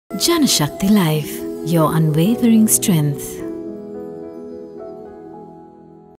The Unwavering Strength in Life
A mother loves us from the first minute of our birth till her last breath. Her love for us transcends prejudice, judgement and doubts and is the strongest bond that gives us the strength to go about our lives. This unwavering strength gives us the motivation to reach for the stars. Throughout the years, Janashakthi has been the source of unwavering strength in lives of countless Sri Lankans and we will strive to remain so in years to come.